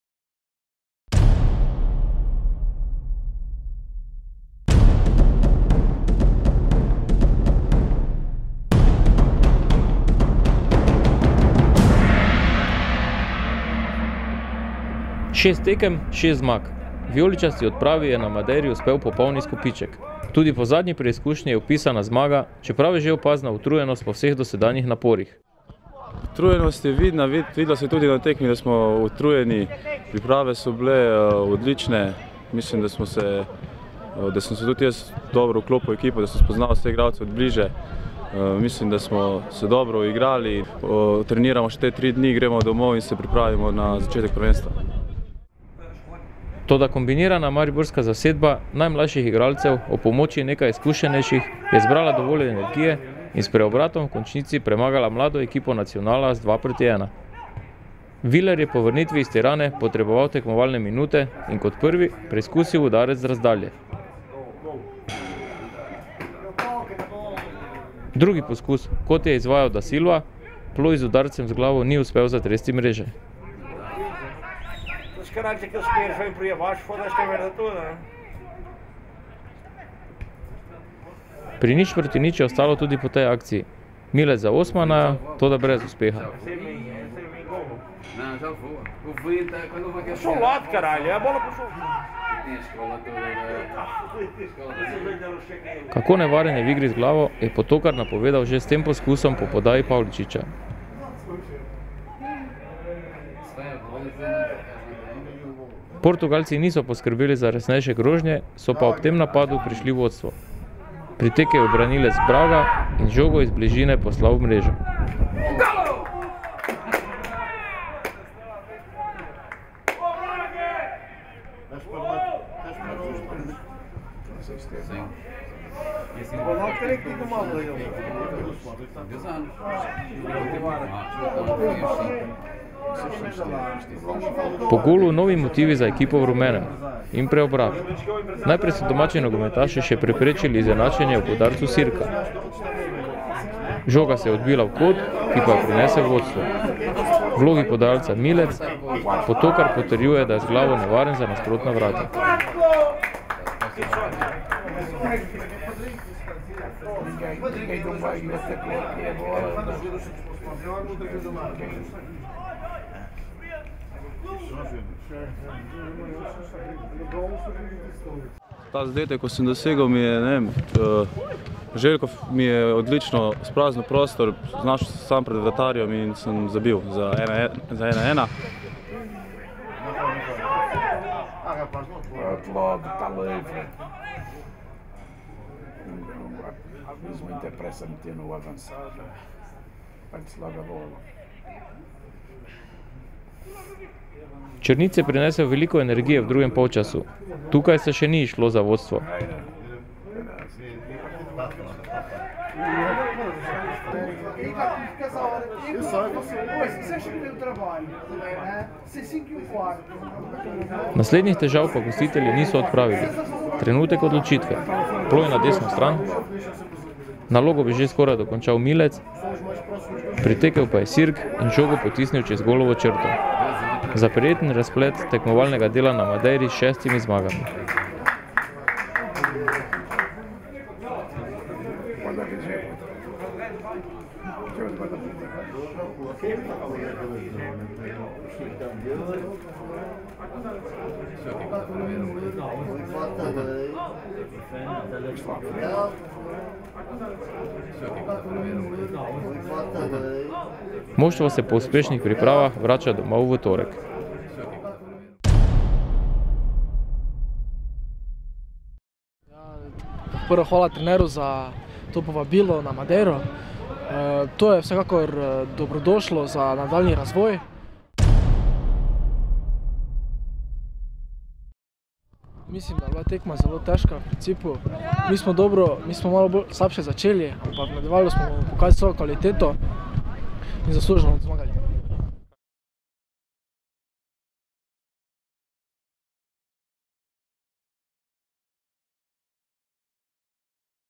ŠEST OD ŠESTIH
Madeira, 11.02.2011 - prijateljska tekma mladih CD Nacional - NK Maribor 1:2